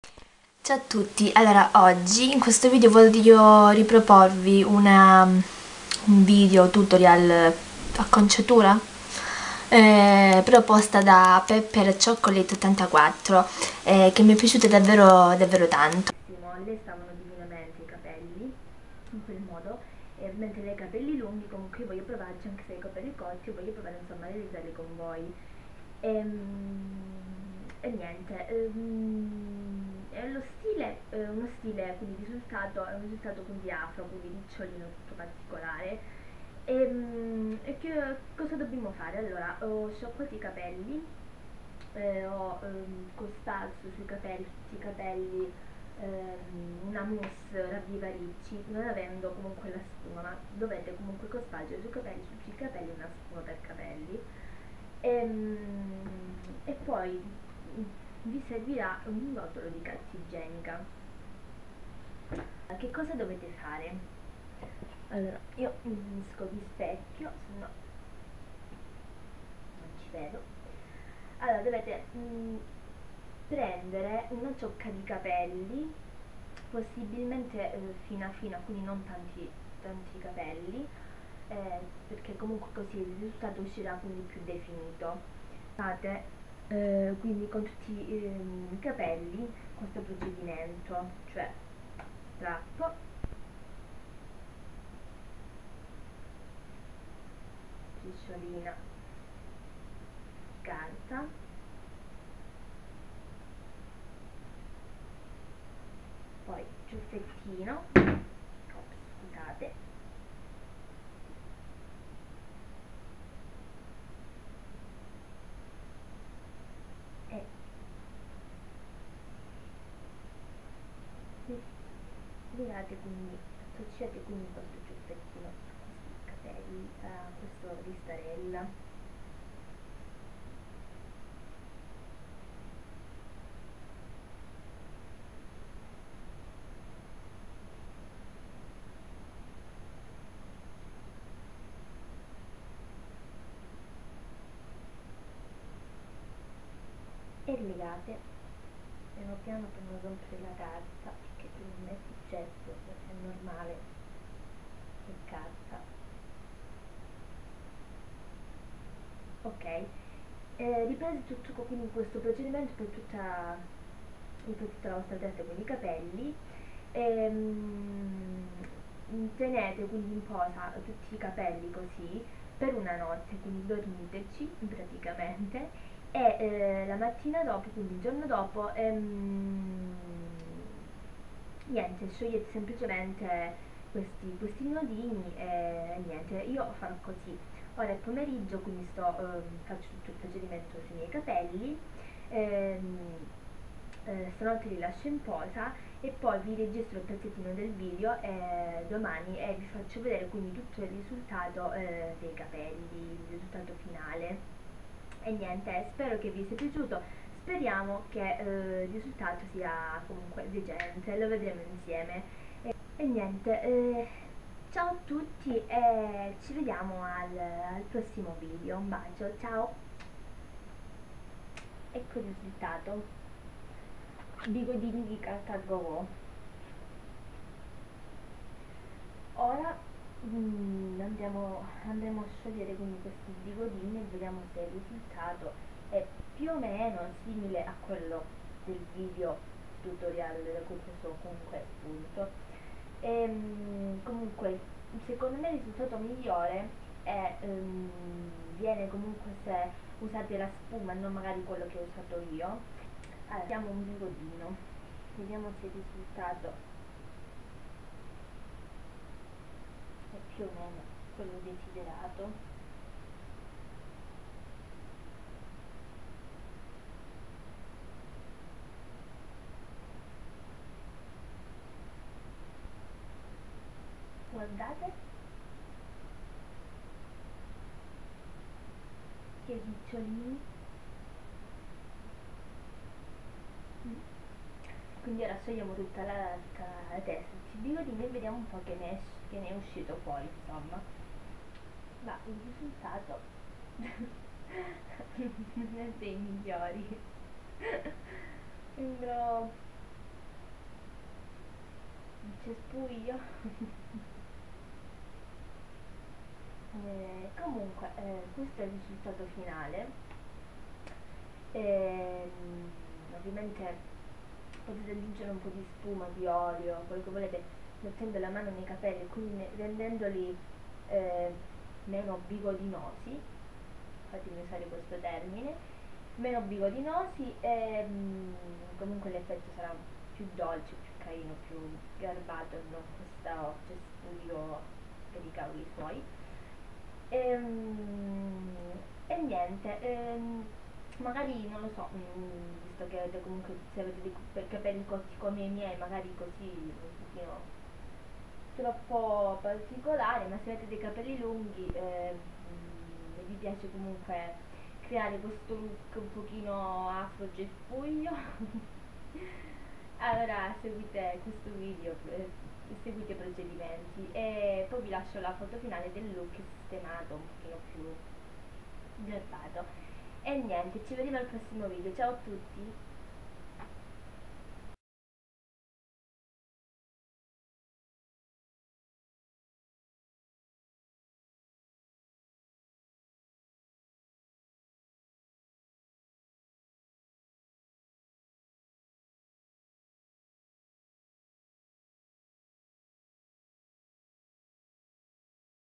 Tutorial Ogni riccio un capriccio
innanzitutto  mi scuso per l'audio ma nonostante le modifiche apportate nell'editing non è migliorato spero che riuscitate comunque a capire vi consiglio di alzare il volume alle stelle ahaha

In questo video tutorial  voglio mostrarvi una proposta di Pepperchocolate84 che mi è piaciuta davvero tanto , ho guardato il suo video tutorial e mi è piaciuto da subito, mi è piaciuto molto il risultato finale e il procedimento diciamolo casalingo e semplice, per ottenere capelli ricci o afro.A  lei stavano divinamente ed ha ottenuto un risultato afro il mio è un risultato diciamo riccioloso  ho deciso di ricrearlo  anche perchè ho sempre voluto i capelli ricci ma con i capelli difficili che ho io è un impresa :) io poi ho un ciuffo laterale ahaha.
 Spero vi piaccia , se  lo realizzate fatemi sapere il risultato son curiosa :P

ps. armatevi di carta igienica e pazienza :P